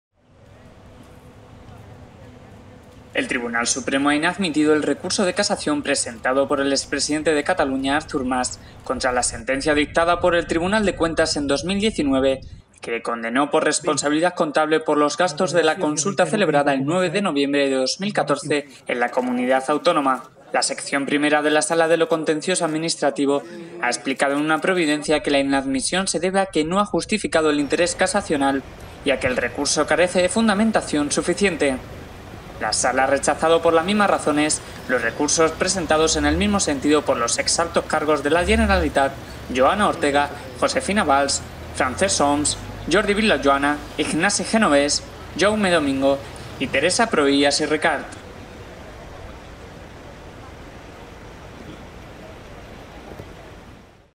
El TS rechaza el recurso de Artur Mas contra la condena del Tribunal de Cuentas
El Tribunal Supremo ha inadmitido el recurso de casación presentado por el ex presidente de Cataluña Artur Mas contra la sentencia dictada por el Tribunal de Cuentas en 2019 que le condenó por responsabilidad contable por los gastos de la consulta celebrada el 9 de noviembre de 2014 en la comunidad autónoma.